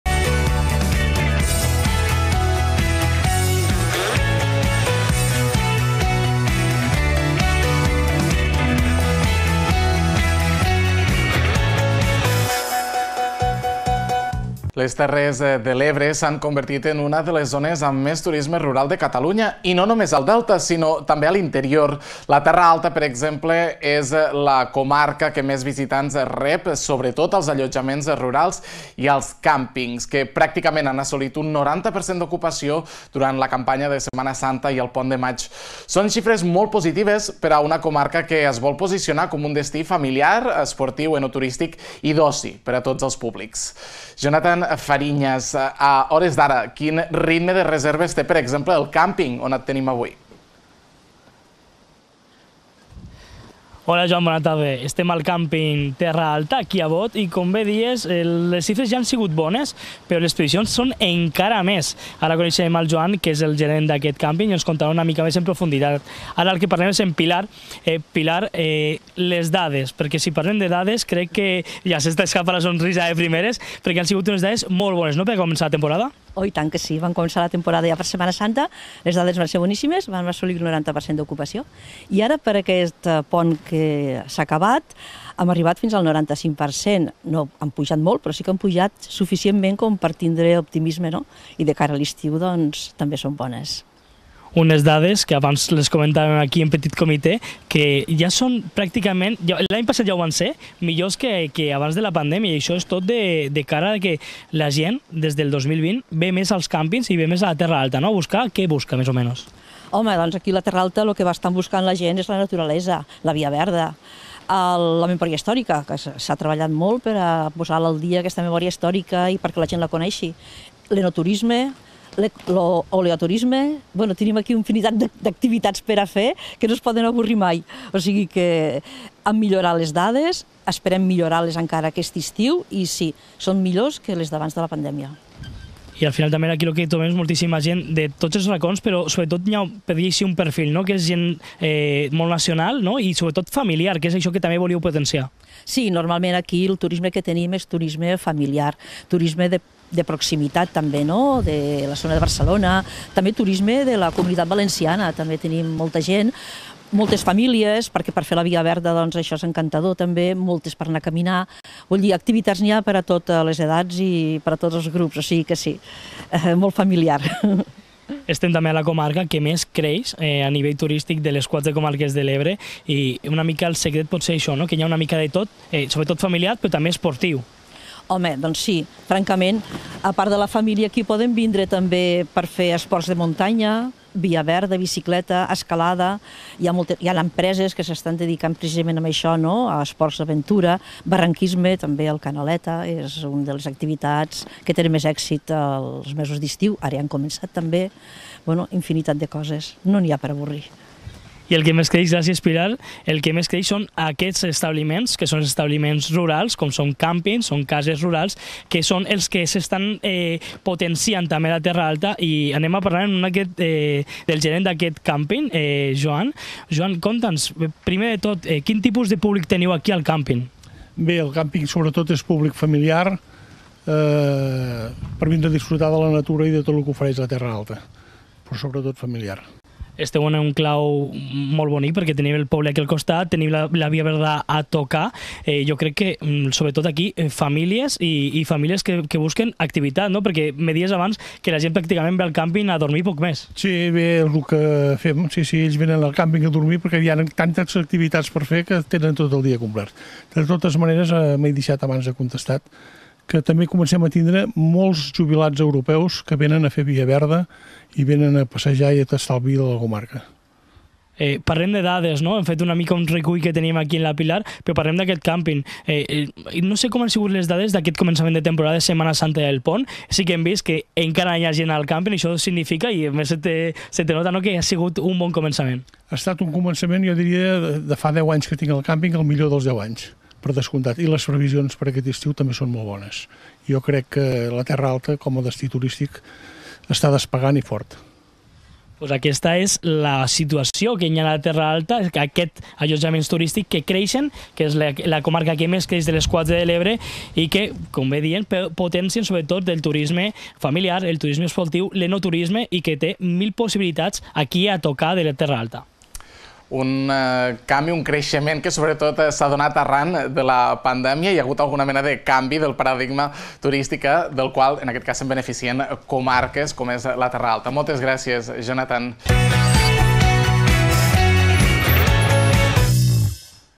C10COMARQUES La Terra Alta obri la temporada turística amb dades d'ocupació record
Les Terres de l'Ebre s'han convertit en una de les zones amb més turisme rural de Catalunya, i no només el Delta sinó també les zones d'interior. La Terra Alta en concret, és la comarca que més creix en ocupació, sobretot en allotjaments rurals i càmpings que pràcticament han assolit un 90% en l'inici de la temporada amb la Setmana Santa i aquest pont de maig. Unes xifres de visitants a una comarca que aposta i ja es posiciona com un destí familiar, esportiu, enoturístic i d'oci per a tots els públics i amb una gran oferta d'activitats.